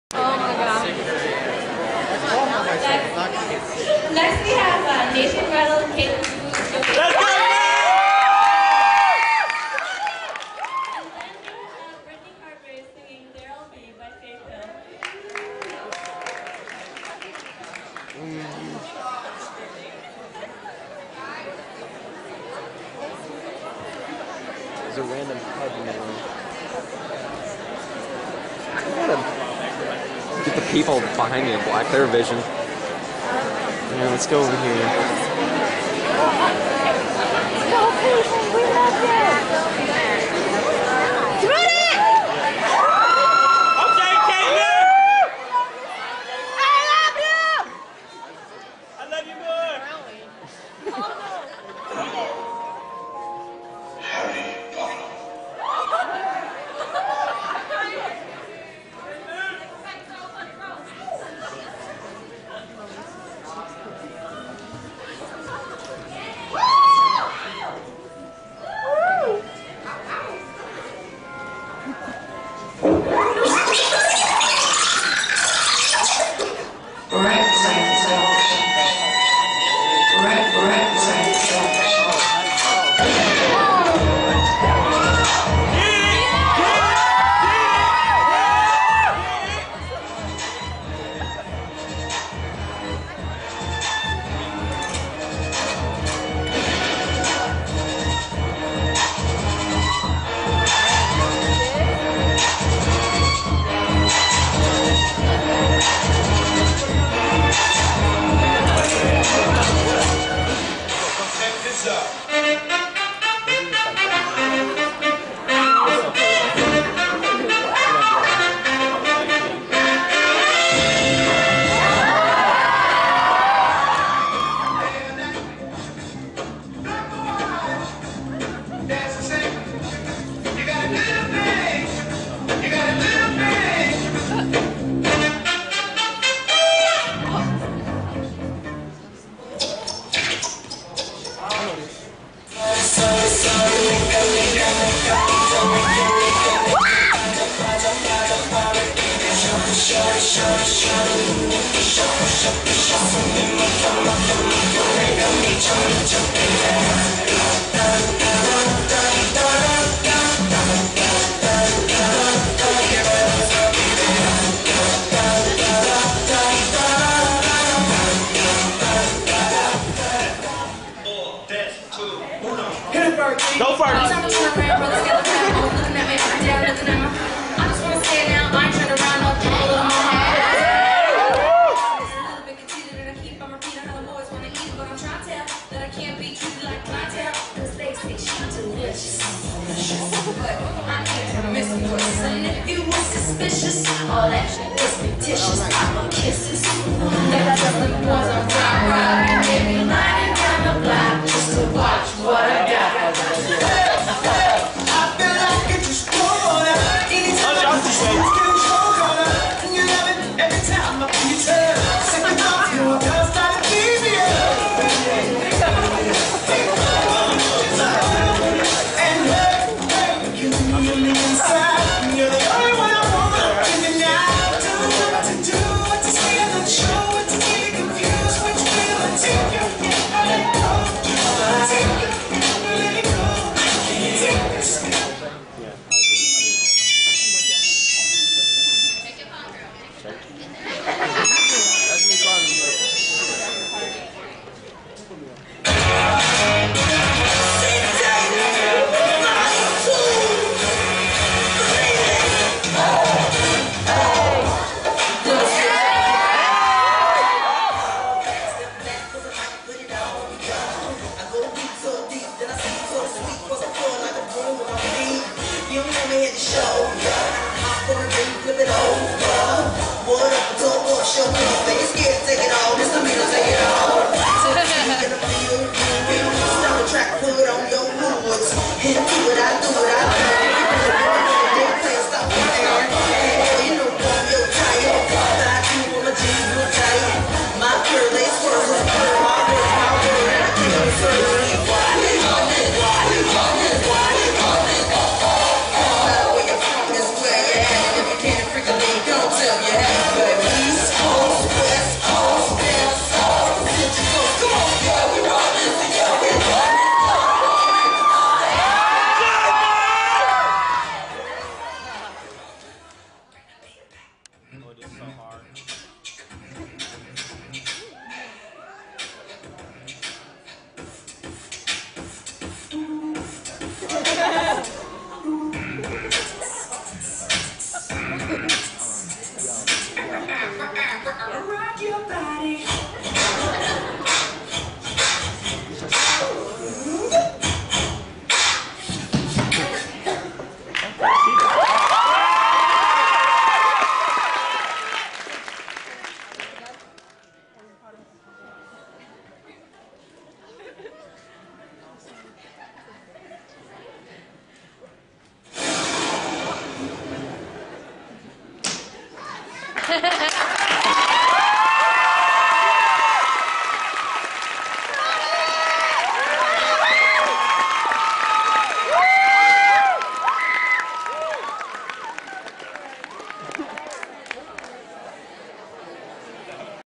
Funky Harry Potter routine (Grand Blanc High School Talent Show)
2010 GBHS Talent Show, Nathan and I.
Probably one of my greatest memories from High School, and one of the most fun routines I've ever done. I will never forget this night.